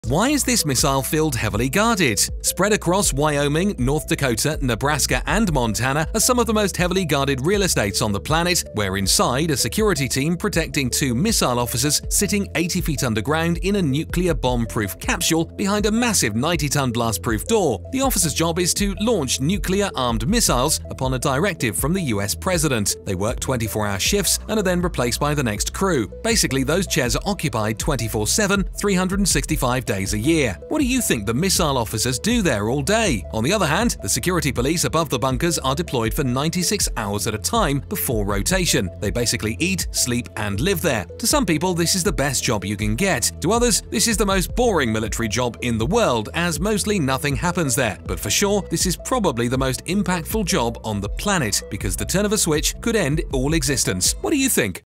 Why Is This Missile Field Heavily Guarded?
Spread across Wyoming, North Dakota, Nebraska and Montana are some of the most heavily guarded real estates on the planet where inside a security team protecting two missile officers sitting 80 feet underground in a nuclear bomb -proof capsule behind a massive 90 -ton blast-proof door. The officer’s job is to launch nuclear -armed missiles upon a directive from the U.S. president. They work 24 -hour shifts and are then replaced by the next crew. Basically, those chairs are occupied 24/7 - 365 days a year. What do you think the missile officers do there all day?
On the other hand, The security police above the bunkers are deployed for 96 hours at a time before rotation. They basically eat, sleep, and live there. To some people, this is the best job you can get. To others, this is the most boring military job in the world as mostly nothing happens there. But for sure, This is probably the most impactful job on the planet, because the turn of a switch could end all existence.

ADDITIONAL READINGS:
A missile launch facility, also known as an underground missile silo, launch facility (LF), or nuclear silo, is a vertical cylindrical structure constructed underground, for the storage and launching of intercontinental ballistic missiles (ICBMs), intermediate-range ballistic missiles (IRBMs), medium-range ballistic missiles (MRBMs). Similar facilities can be used for anti-ballistic missiles (ABMs).

The structures typically have the missile some distance below ground, protected by a large "blast door" on top. They are usually connected, physically and/or electronically, to a missile launch control center.

With the introduction of the Soviet UR-100 and the U.S. Titan II missile series, underground silos changed in the 1960s. Both missile series introduced the use of hypergolic propellant, which could be stored in the missiles, allowing for rapid launches. Both countries' liquid-fueled missile systems were moved into underground silos. The introduction of solid fuel systems, in the later 1960s, made the silo moving and launching even easier.

The underground missile silo has remained the primary missile basing system and launch facility for land-based missiles since the 1960s. The increased accuracy of inertial guidance systems has rendered them somewhat more vulnerable than they were in the 1960s[citation needed].

Other than underground facilities, ballistic missiles can be launched from above-ground facilities, or can be launched from mobile platforms, e.g. transporter erector launchers, railcars, ballistic missile submarines or airplanes.

United States
The British idea of an underground missile silo was adopted and developed by the United States for missile launch facilities for its intercontinental ballistic missiles. Most silos were based in Colorado, Nebraska, North Dakota, South Dakota, Missouri, Montana, Wyoming and other western states. There were three main reasons behind this siting: reducing the flight trajectory between the United States and the Soviet Union, since the missiles would travel north over Canada and the North Pole; increasing the flight trajectory from SLBMs on either seaboard, giving the silos more warning time in the event of a nuclear war; and locating obvious targets as far away as possible from major population centres. They had many defense systems to keep out intruders and other defense systems to prevent destruction (see Safeguard Program). In addition to the three previously mentioned siting reasons, the US Air Force had other site requirements that were also taken into account such as, having the sites be close enough to a populace of roughly 50,000 people for community support along with making sure launch locations were far enough apart that a 10 MT detonation on or near strategic locations would not knock out other launch facilities in the area. "In 1960 the US Army established the Corps of Engineers Ballistic Missile Construction Office (CEBMCO), an independent organization under the Chief of Engineers, to supervise construction". This newly established organization was able to produce Minutemen Launch silos at an extremely fast rate of ~1.8 per day from 1961 to 1966 where they built a total of 1,000 Minuteman missile silos.

The United States built many missile silos in the Midwest, away from populated areas. Many were built in Colorado, Nebraska, South Dakota, and North Dakota. The U.S. spent considerable effort and funds in the 1970s and 1980s designing a replacement, but none of the new and complex system designs were ever produced.

The United States has many silo-based warheads in service, however, they have lowered their number to around 1800 and have transferred most of their missiles to nuclear submarines.